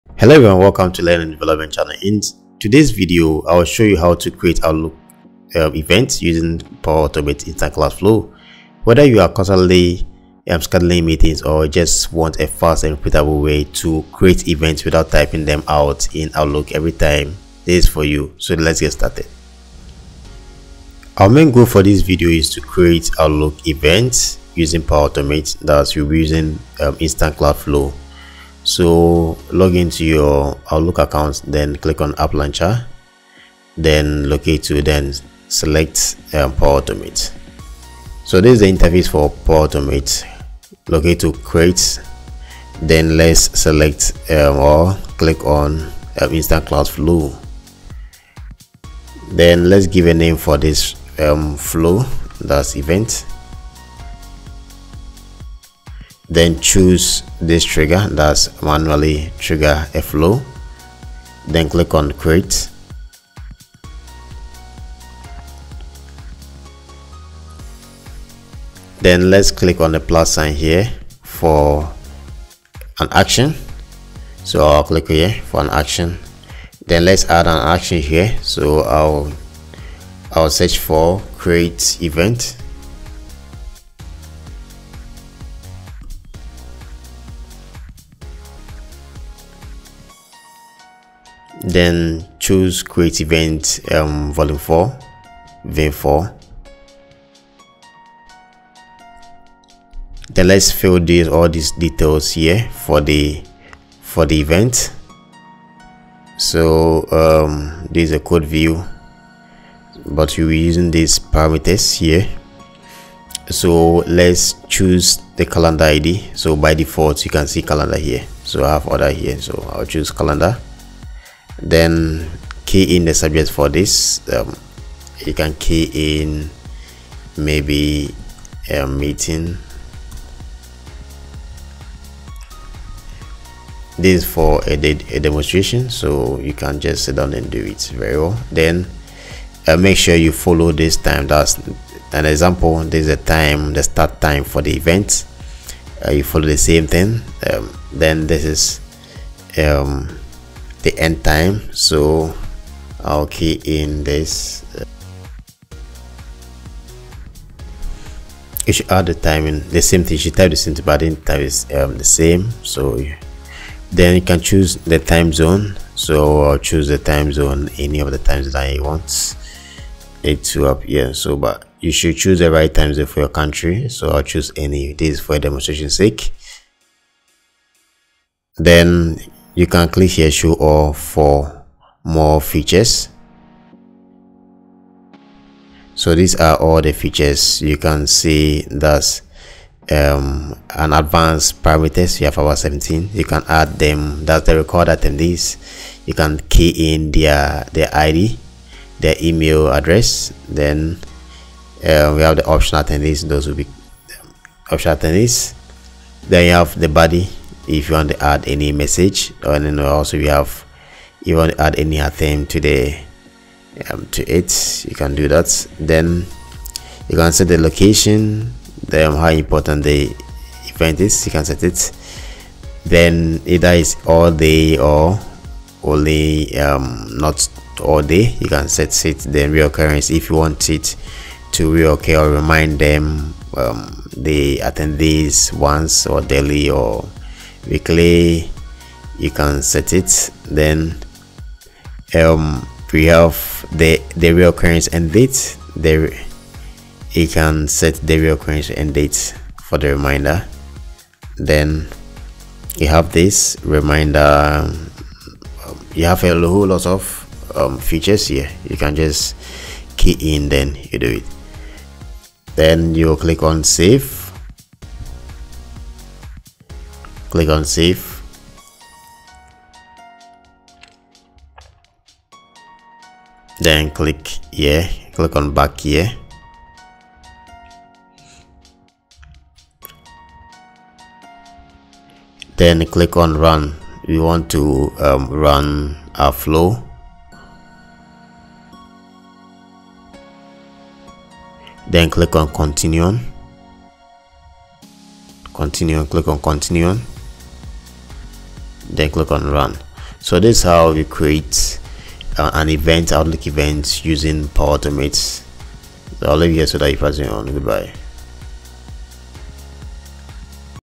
Creating events in Outlook using Power Automate | Weekly Tips #13
📌 Description:

In this step-by-step tutorial, you will learn how to create Outlook calendar events using Power Automate's Instant Cloud Flow. Whether you're manually scheduling meetings, organizing reminders, or streamlining recurring tasks — this guide will show you how to automate it all with just a few clicks.

🔧 What You’ll Learn:

How to create an Instant Cloud Flow in Power Automate

How to add manual inputs like Subject, Time, and Location

How to configure the “Create Event (V4)” action in Outlook

Tips for testing, saving, and reusing your flow for quick event creation